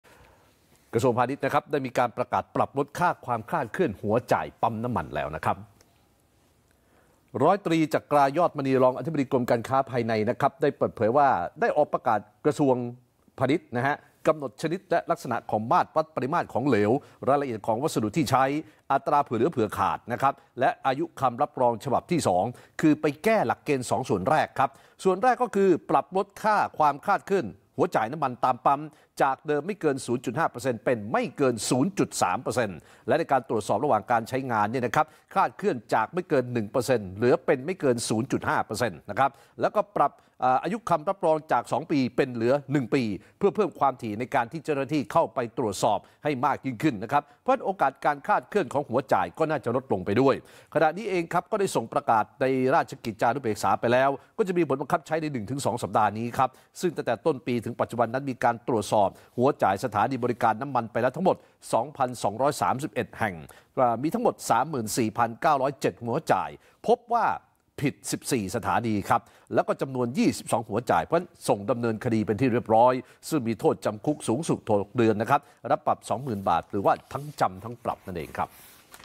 เคาะแล้ว ปรับลดค่าความคลาดเคลื่อนหัวจ่ายปั๊มน้ำมัน ต้องไม่เกิน 0.3% ฝ่าฝืนคุก 6 เดือน
กระทรวงพาณิชย์ ประกาศปรับลดค่าความคลาดเคลื่อนหัวจ่ายปั้มน้ำมัน ต้องไม่เกิน 0.3% ฝ่าฝืนคุก 6 เดือน

ร้อยตรีจักรา ยอดมณี รองอธิบดีกรมการค้าภายใน เปิดเผยว่า ได้ออกประกาศกระทรวงพาณิชย์ กำหนดชนิดและลักษณะของมาตรวัด ปริมาตรของเหลว รายละเอียดของวัสดุที่ใช้ผลิต อัตราเผื่อเหลือเผื่อขาด และอายุคำรับรอง ฉบับที่ 2 โดยปรับแก้หลักเกณฑ์ 2 ส่วน ส่วนแรก คือ การปรับลดค่าความคลาดเคลื่อนหัวจ่ายน้ำมัน จากเดิมไม่เกิน0.5% เป็นไม่เกิน 0.3% และในการตรวจสอบระหว่างการใช้งาน จากไม่เกิน 1% เป็นไม่เกิน 0.5% พร้อมปรับอายุคำรับรองจาก 2 ปี เป็น 1 ปี ซึ่งเป็นการเพิ่มความถี่ในการตรวจสอบให้มากขึ้น โอกาสที่จะเกิดความคลาดเคลื่อนก็จะลดลงไปด้วย โดยขณะนี้ได้ส่งไปประกาศในราชกิจจานุเบกษาแล้ว คาดว่าจะมีผลบังคับใช้ใน 1-2 สัปดาห์นี้ ซึ่งตั้งแต่ต้นปีถึงปัจจุบัน มีการตรวจสอบหัวจ่ายสถานีบริการน้ำมันแล้ว 2,231 สถานี จำนวน 34,907 หัวจ่าย พบว่าผิด 14 สถานี จำนวน 22 หัวจ่าย ซึ่งได้ส่งดำเนินคดีแล้ว ซึ่งมีโทษจำคุกสูงสุด 6 เดือน ปรับ 20,000 บาท หรือทั้งจำทั้งปรับ

เรื่องเด่นเย็นนี้ วันที่ 16 กุมภาพันธ์  2567